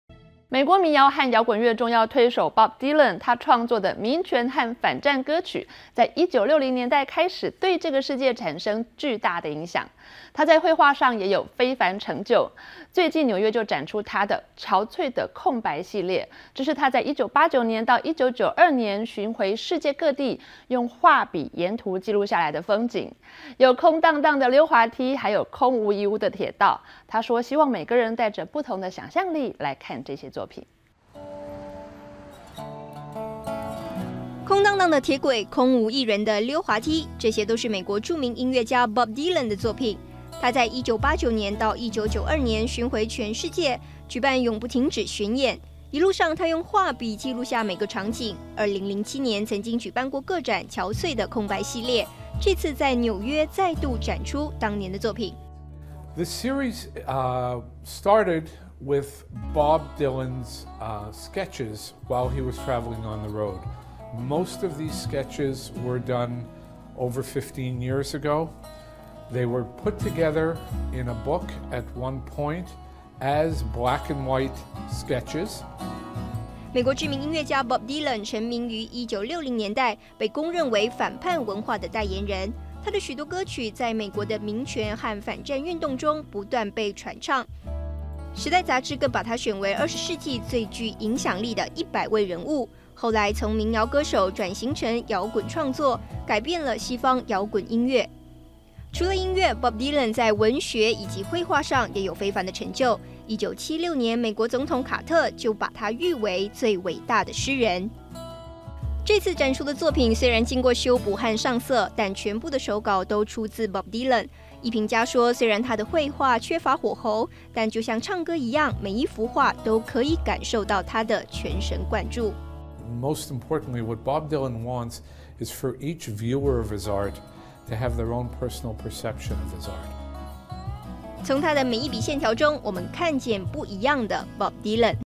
20140507《藝想世界》民謠之父Bob Dylan畫展 《憔悴的空白系列》
美國民謠和搖滾樂重要推手Bob Dylan，他創作的民權和反戰歌曲，在1960年代開始，對這個世界產生巨大的影響。他在文學和繪畫上也有非凡成就。紐約最近展出他的《憔悴的空白系列》，這是他在1989年到1992年，巡演世界各地時，用畫筆沿途記錄下的風景。有空蕩蕩的溜滑梯，還有空無一物的鐵道。他說希望每個人都帶著不同的想像力來看這些作品。